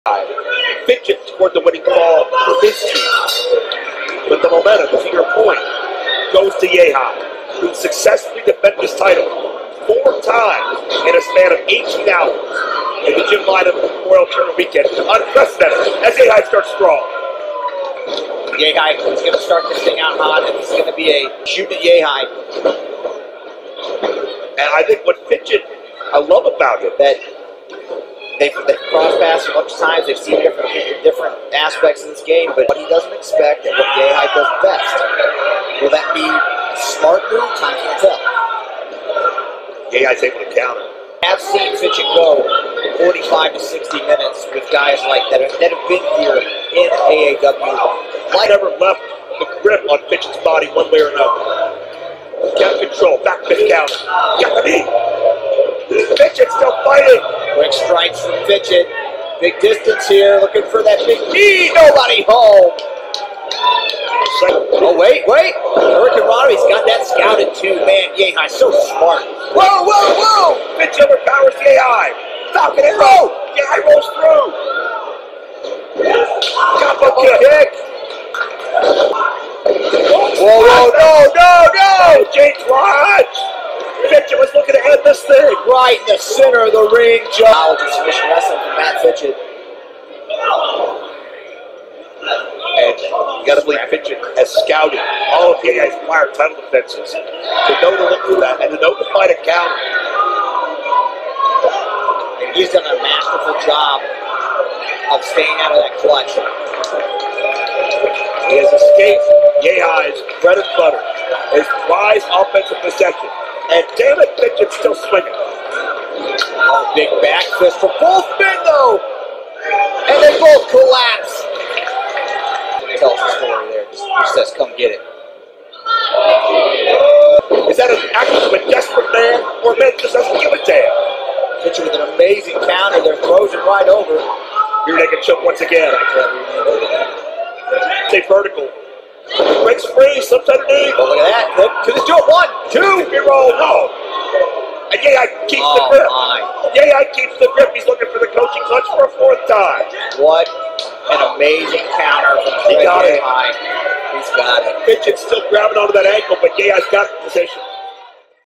Fred Yehi vs Mat Fitchett Highlights | AAW | Windy City Classic | 2021
Yehi defends AAW Title against Matt Fitchett